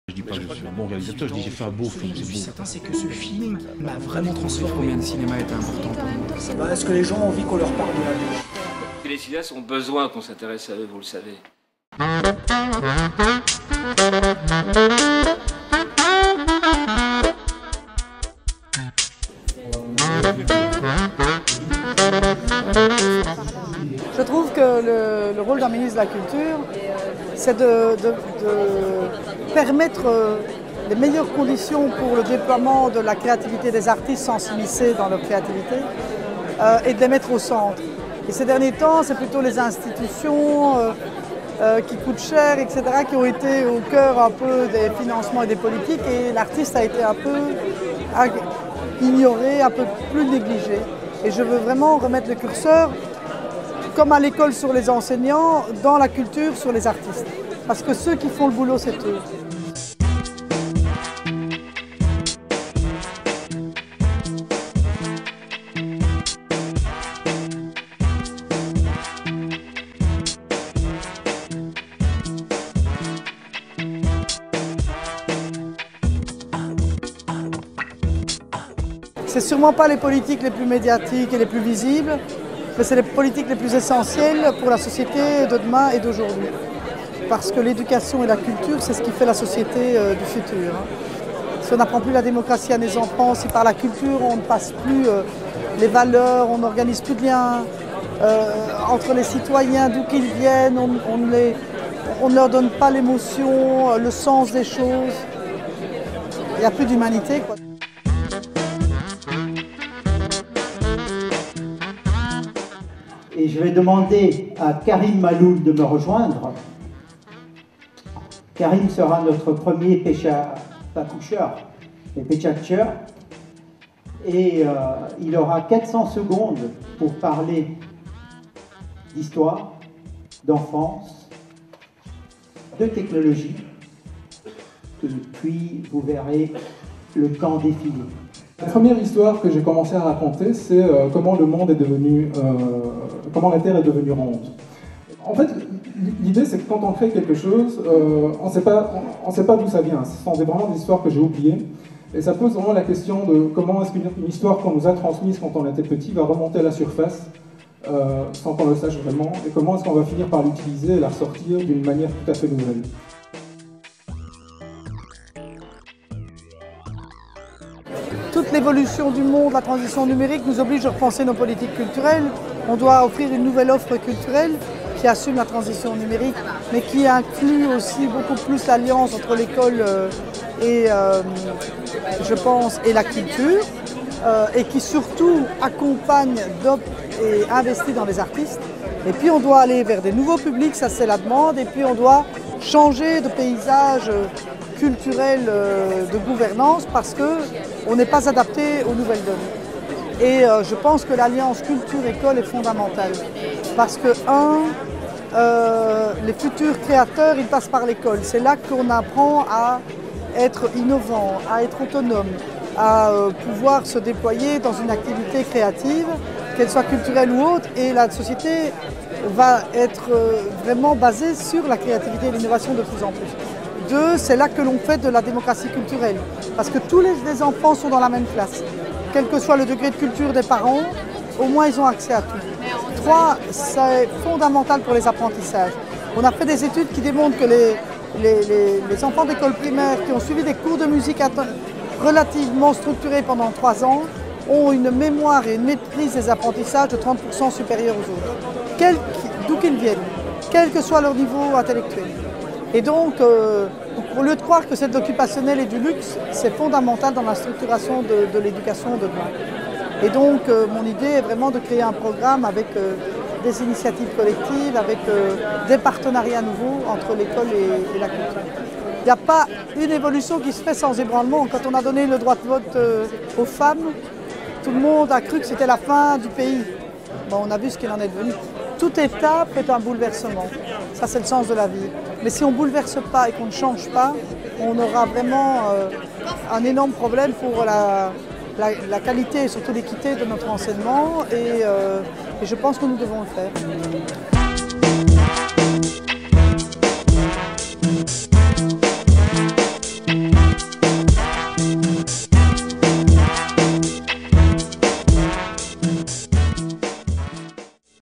Interview de Joëlle Milquet, ministre de la culture et de l'éducation
Cinergie s'est brièvement entretenu avec la ministre de la culture, Joëlle Milquet, lors de la présentation de son programme culturel "Bouger les lignes", au Théâtre National le 19 janvier dernier.